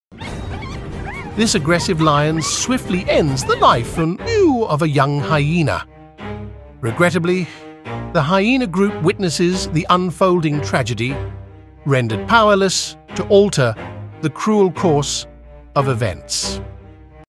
Survival Showdown: Lion's Audacious Hunt Young Hyena | Leaves Hyenas Speechless!
Brace yourself for an adrenaline-pumping faceoff in the wild! Watch as a fearless lion takes on the unexpected challenge of hunting a young hyena, leaving the entire hyena clan in shock. Nature's survival drama unfolds in this jaw-dropping encounter, where the hunter becomes the hunted, and the hyenas are left in stunned silence. Don't miss this extraordinary moment – click now to witness the raw power and unpredictability of the animal kingdom!